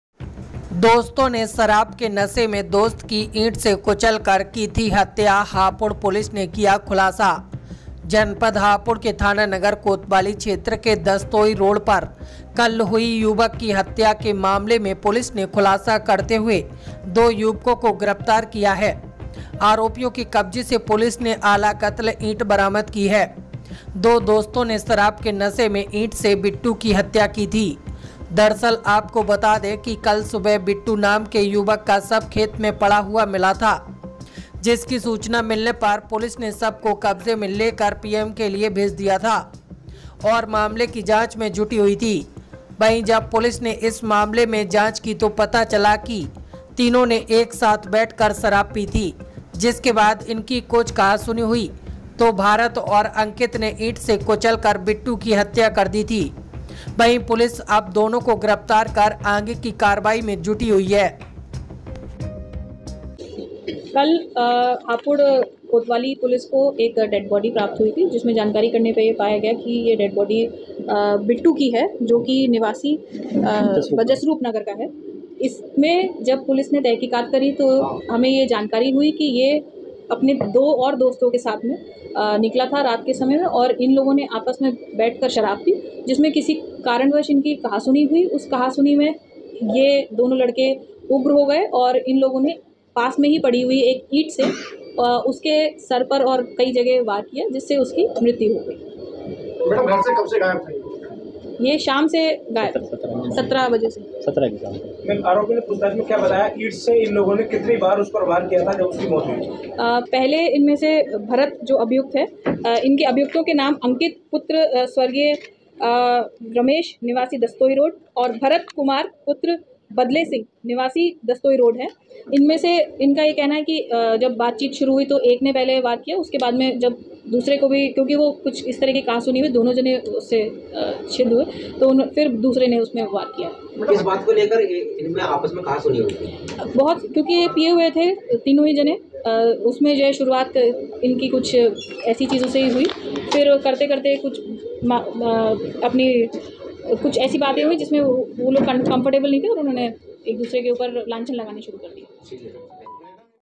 दो दोस्तों ने ईंट से की थी दोस्त की **या, Hapur Police ने किया खुलासा
MSS न्यूज़

रिपोर्ट- सुनील कुमार

हापुड़ यूपी

दो दोस्तों ने ईंट से की थी दोस्त की हत्या, हापुड़ पुलिस ने किया खुलासा

जनपद हापुड़ के थाना नगर कोतवाली क्षेत्र के दस्तोंई रोड़ पर कल हुई युवक की हत्या के मामले में पुलिस ने खुलासा करते हुए दो युवको को किया गिरफ़्तार ।

 आरोपियों के क़ब्ज़े से पुलिस ने आलाक़त्ल ईंट बरामद की है, दो दोस्तों ने शराब के नशे में ईंट से बिट्टू की हत्या की थी,
दरअसल आपको बता दें कि कल सुबह बिट्टू नाम के युवक का शव खेत में पड़ा मिला था, जिसकी सूचना मिलने पर पुलिस ने शव को क़ब्ज़े में लेकर पीएम के लिए भेज दिया था और मामले की जाँच में जुट हुई थी, वही जब पुलिस ने इस मामले में जाँच की तो पता चल की तीनों ने एक साथ बैठकर शराब पी थी जिसके बाद इनकी कुछ कहा सुनी हुई तो भारत और अंकित ने ईंट से कुचलकर बिट्टू की हत्या कर दी थी, वही पुलिस अब दोनों को गिरफ़्तार कर आगे की कार्यवाही में जुटी हुई है।

Join & Support MSS NEWS 24X7 News Channel -